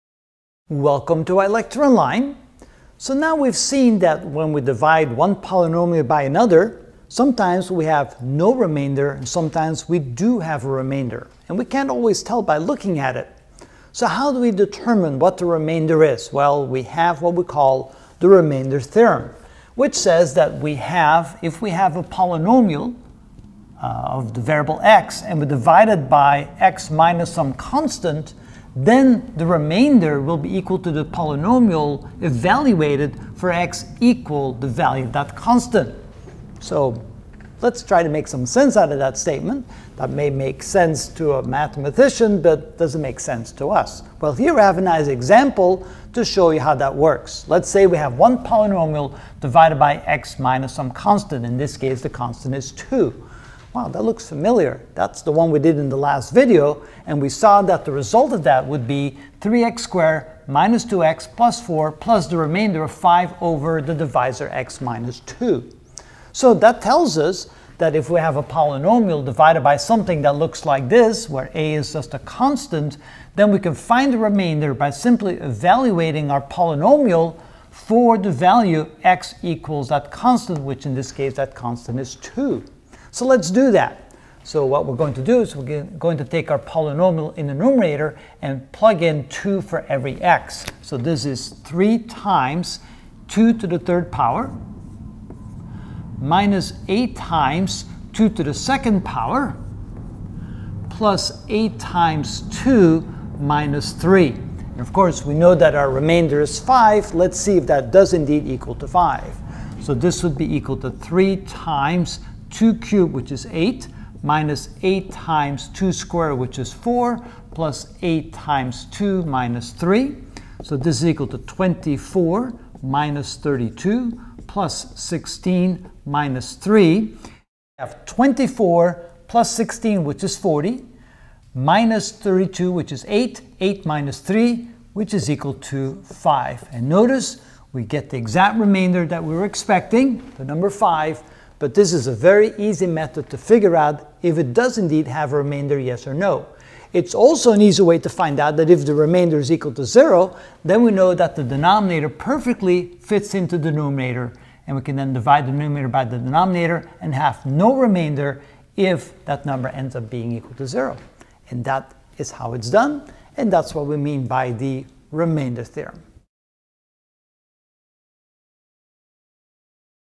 Algebra - Ch. 10: Rational Expression (24 of 33) What is the Remainder Theorem?
In this video I will explain by example of what is the Remainder Theorem.